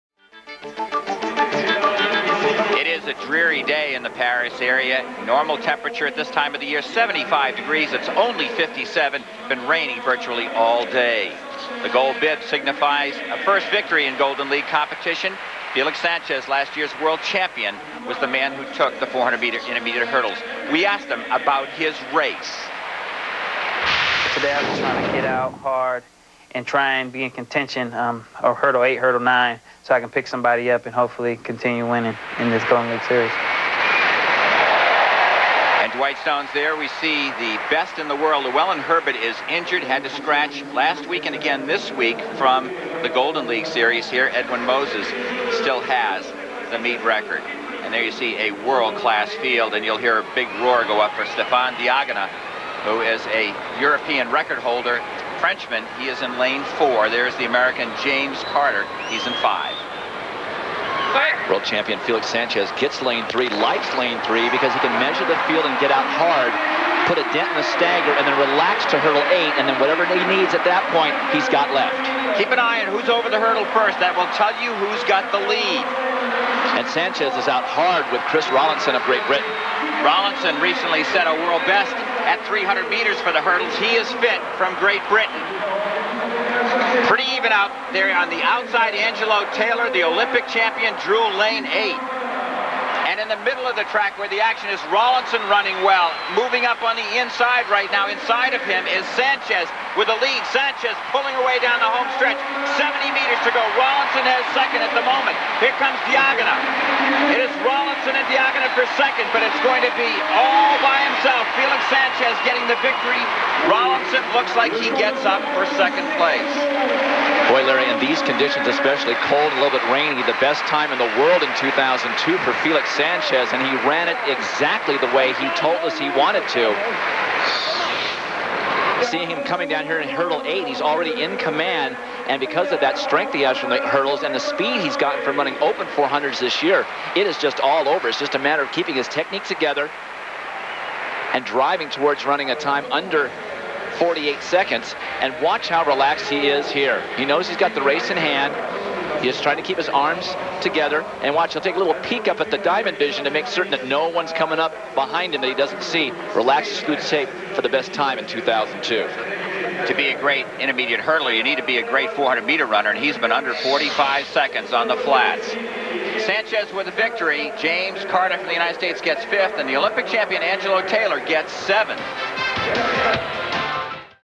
Felix Sánchez - Men's 400m Hurdles - 2002 Paris Grand Prix
Gaz de France (Grand Prix)
Paris, France
July 5, 2002
Men’s 400m Hurdles Results:
     1) Felix Sánchez – 47.91
     2) Christopher Rowlinson – 48.26
     3) Stéphane Diagana – 48.28
     4) Mori Fabrizio – 48.49
     5) James Carter – 48.54
     6) Dai Tamesue – 48.88
     7) Angelo Taylor – 49.28
     8) Alwyn Myburgh – 49.49